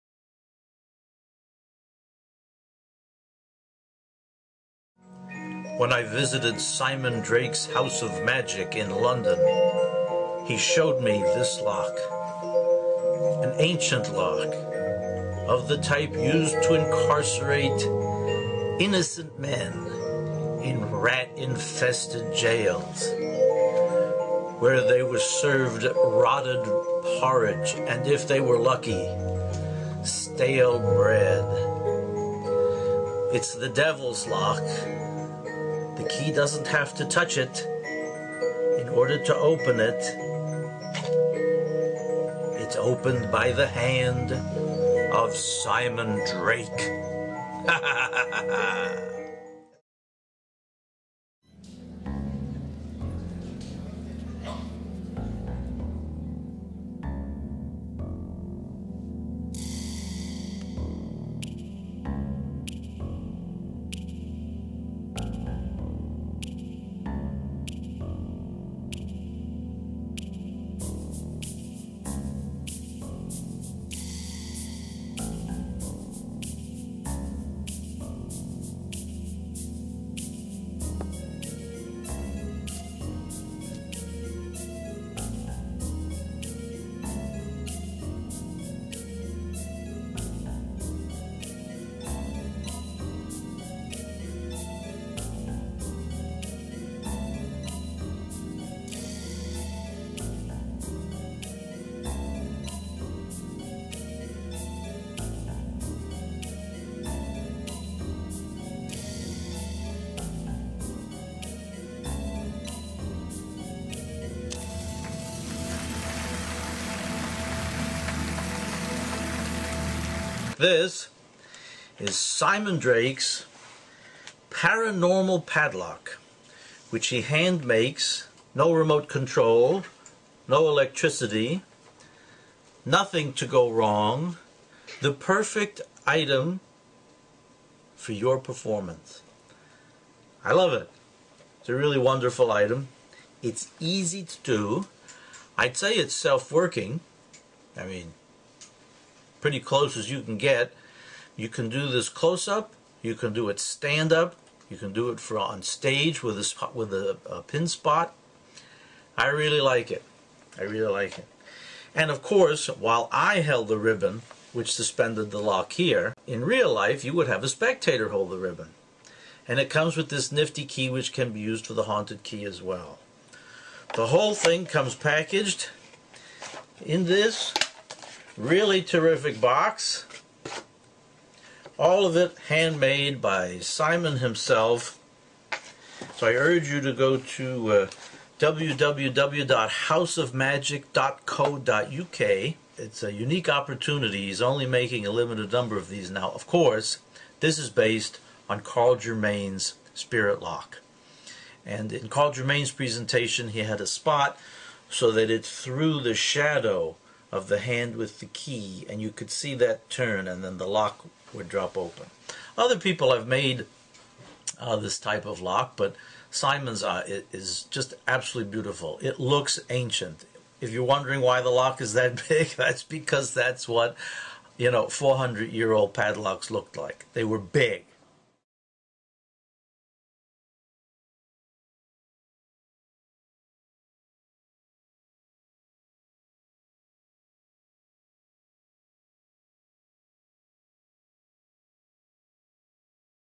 Simon Drake's version of Germain's Spirit Lock with Haunted key effect
Simon Drake's version of Germain's Spirit Lock with review by highly respected editor in chief of Genii Magazine, Mr Richard Kaufman. Genii is the oldest and biggest magazine for magic and illusion on the planet. Also features the Haunted key effect. Thank you Richard!